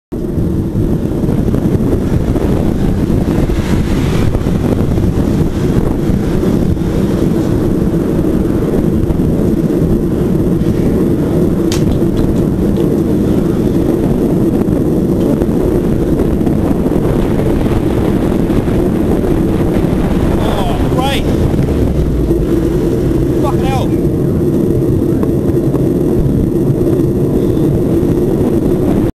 Deni International Foods truck driver cannot wait!
Despite the empty road behind me, this truck driver just couldn't wait to turn onto King St.